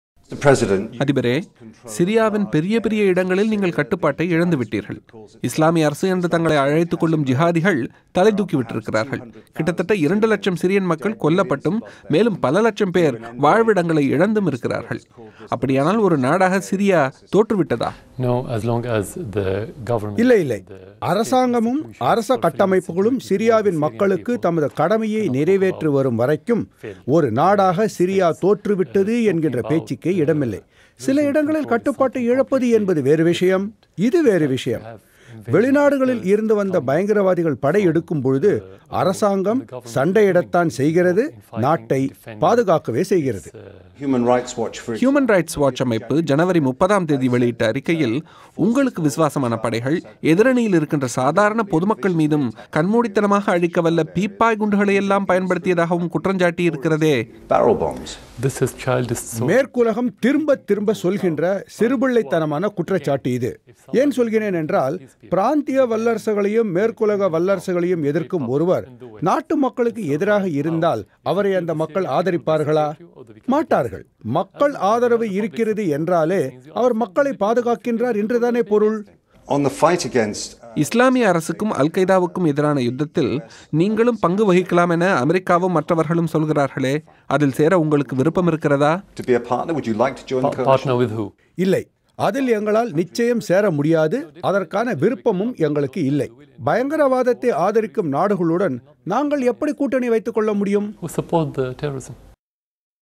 பொதுமக்களை கண்மூடித்தனமாய்க் கொல்லவில்லை: சிரியாவின் அதிபர் பிரத்யேகப் பேட்டி
சிரியாவின் அதிபர் பஷார் அல் அஸத் பிபிசிக்கு வழங்கிய பிரத்யேகப் பேட்டியில், பீப்பாய் குண்டுகளைப் பயன்படுத்தி சிரியாவின் அரச படைகள் பொதுமக்களைக் கொன்றதாய்த் தெரிவிக்கப்படும் குற்றச்சாட்டுகளை நிராகரித்துள்ளார்.